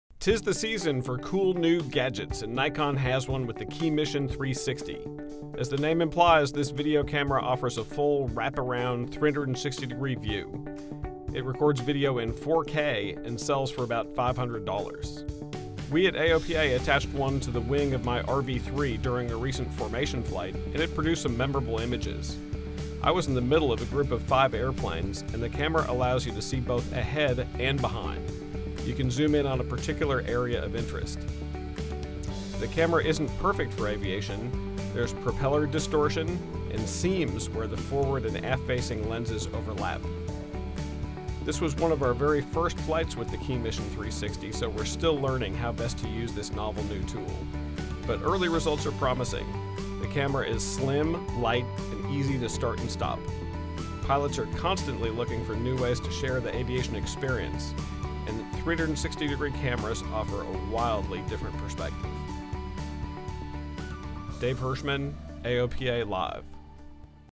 Nikon Keymission 360
We test Nikon's new KeyMission 360 action camera in the air.